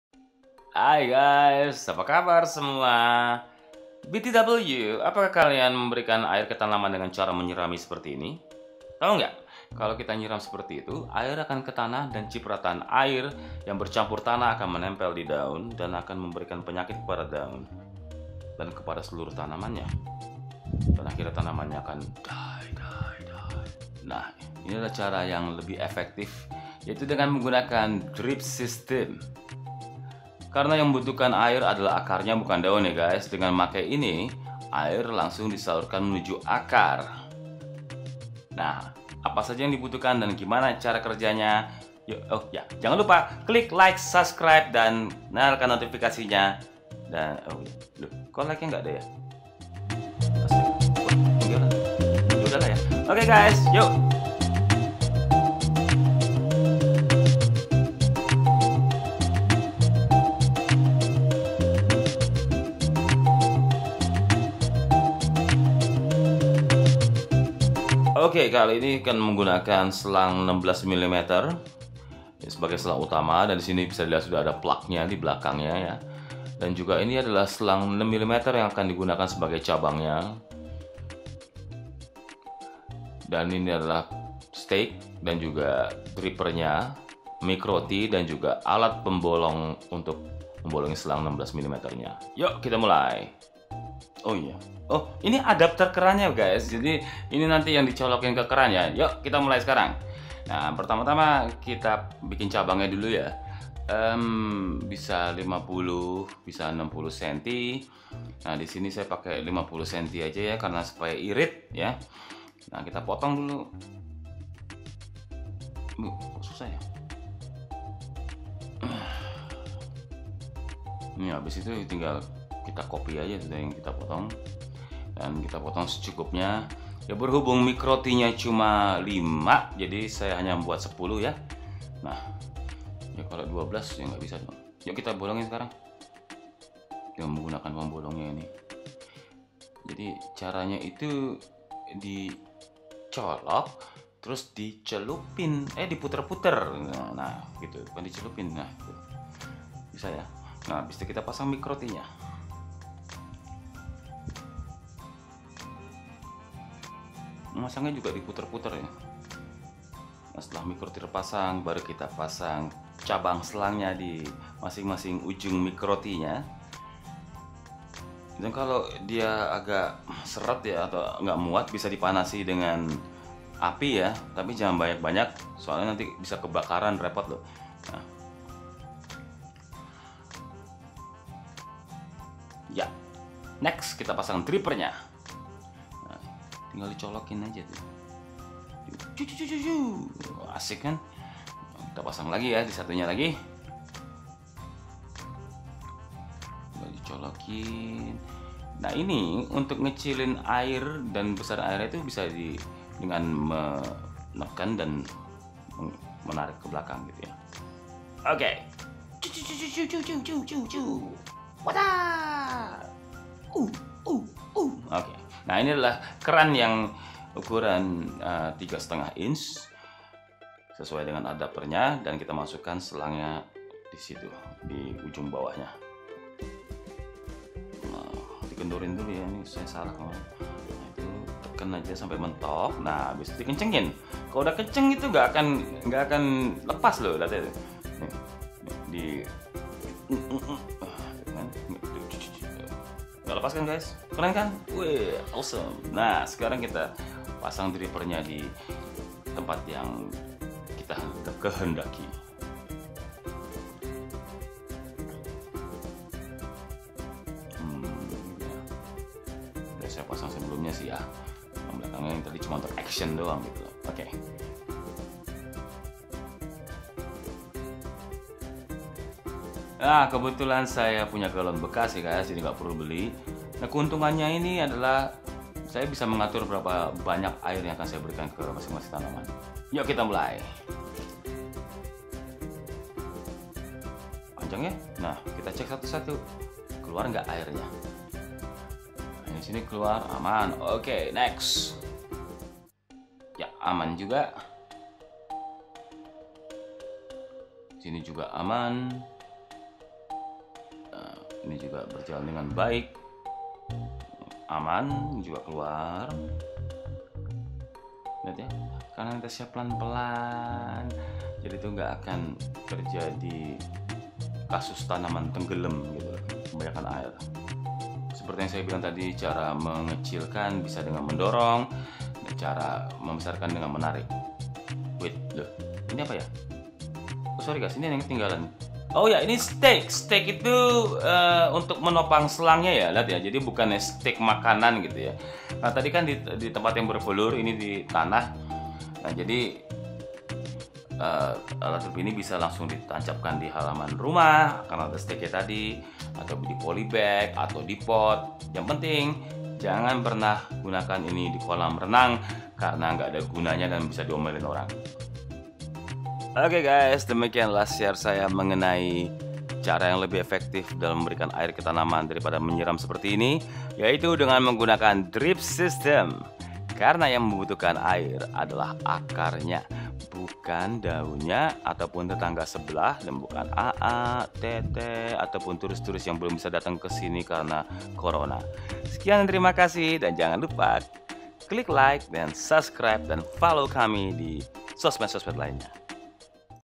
Kegunaan dan cara pemasangan selang irigasi drip sistem skala kebun rumah
kegunaan dan cara pemasangan selang irigasi buat drip pada kebun rumah.